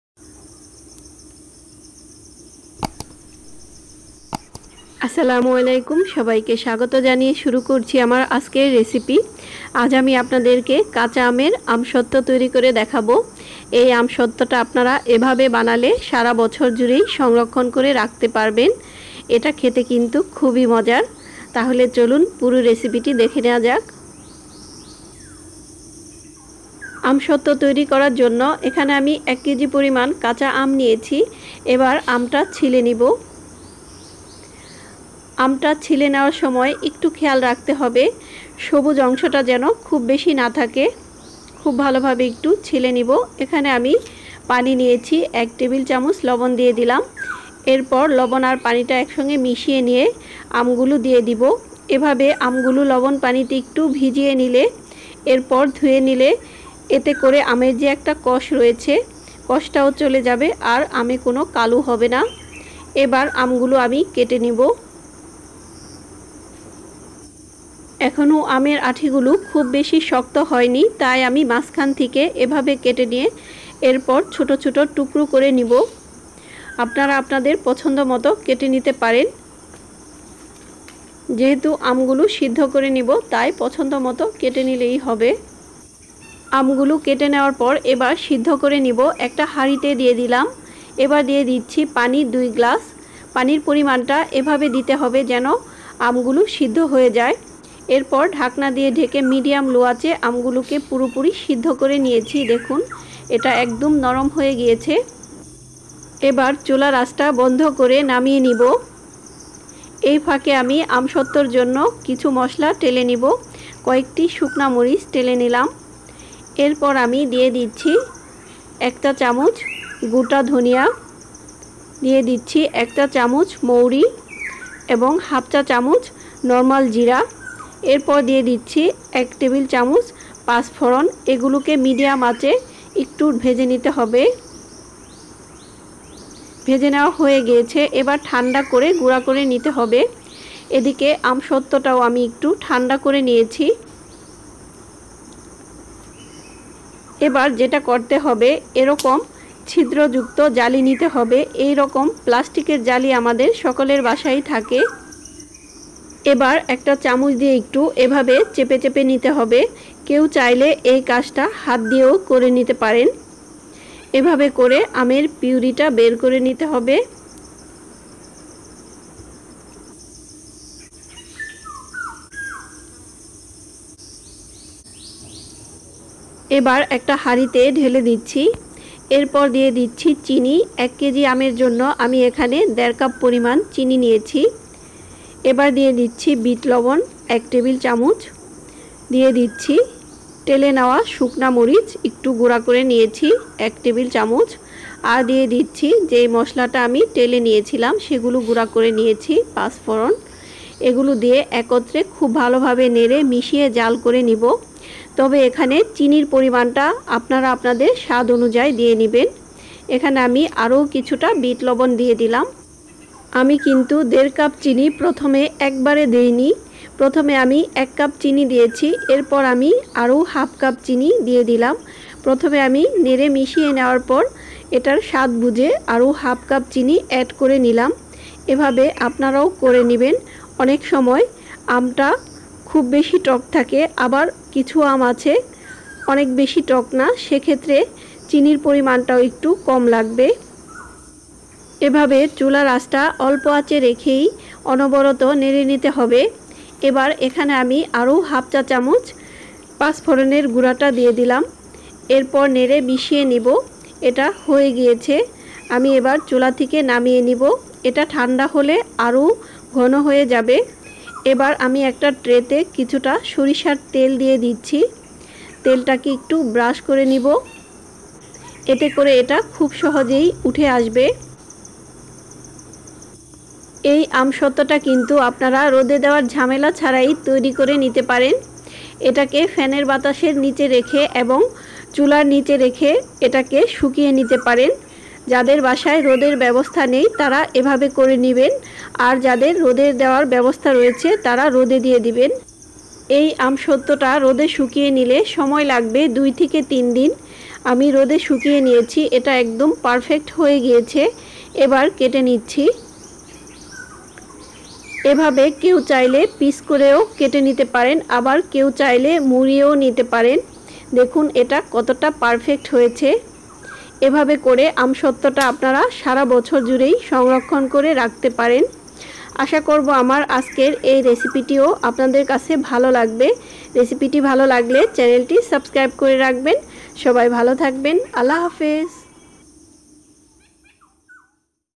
কাঁচা আমের আমসত্ত্ব রেসিপি | Khacha Amer Amshotto
Ingredients :
🔹green mango 1 kg.
🔹sugar 1 cup
🔹red chilli pepper 5-6 pc
🔹pachforon 1 tbsp
🔹whole coriander 1 teaspoon
🔹fennel 1 teaspoon
🔹cumin half teaspoon
🔹bit salt to taste